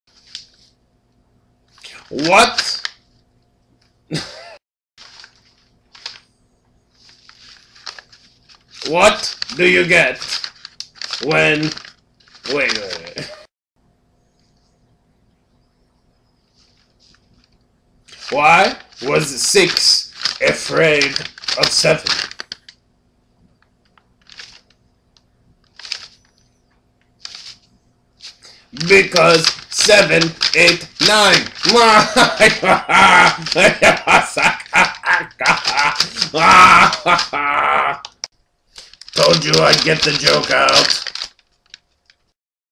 Bagman Tells a Joke
Bagman tries to tell a joke. For DS106's The Daily Create.